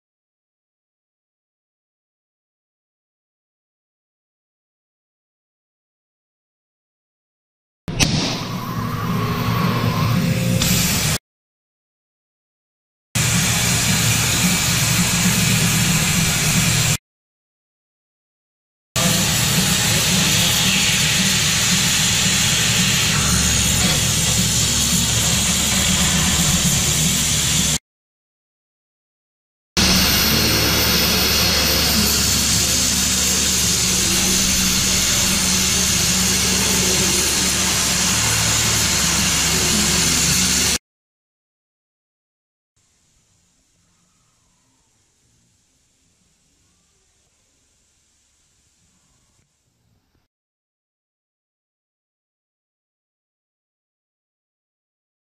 Plazmax AutoGas Oxy-Fuel Cutting System
An optional feature on many of our models, this high quality automatic oxy-fuel cutting system removes the manual processes come with traditional CNC gas cutting. With the ability to ignite, preheat, pierce, and cut all automated consistent cutting results are easily achieved. The intelligent height sensing capability will detect slag and raise the torch to compensate, reducing the risk of damage to both the plate and the part.